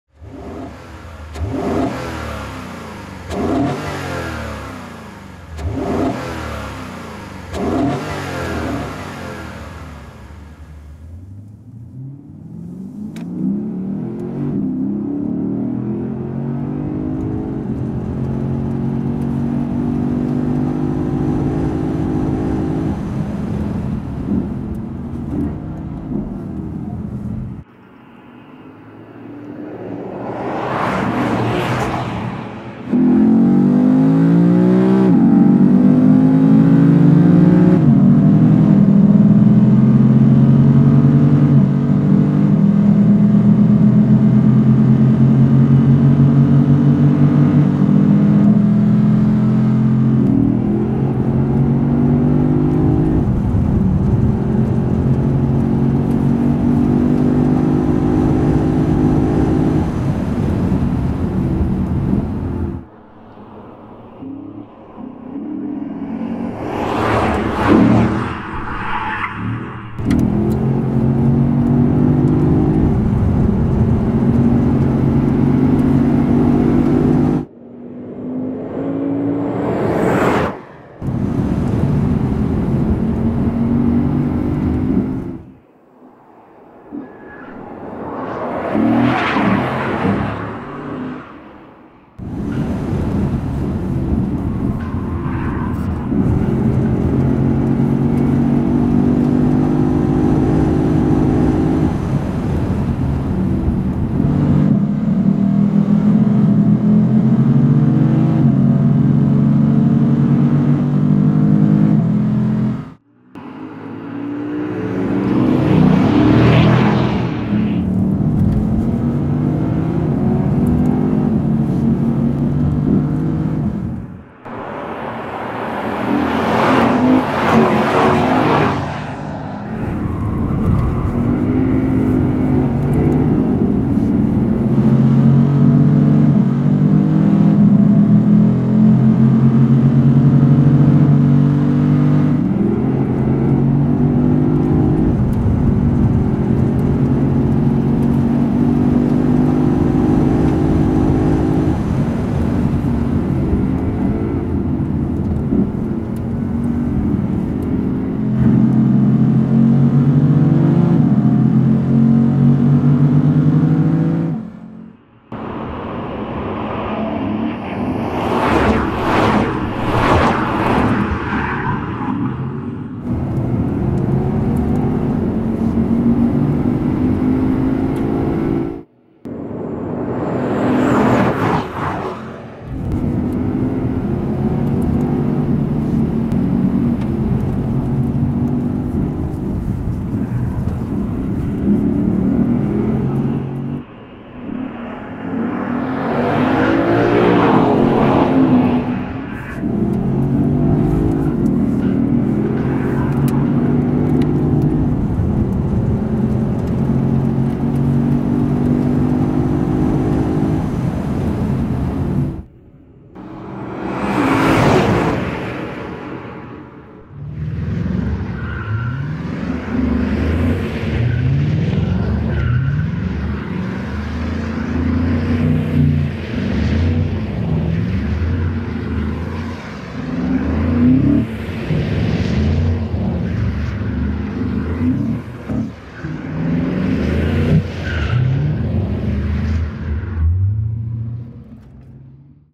Virtual reality (aggressive drive) | Lexus RC F | Lexus Europe
Experience the brutal elegance of the race–tuned RC F in thrilling 360 virtual reality. Join the aggressive race driving experience at the beautiful Ascari racetrack. For the best results please view in Google’s Chrome browser.

Discover more Lexus virtual driving experiences and download the RC F app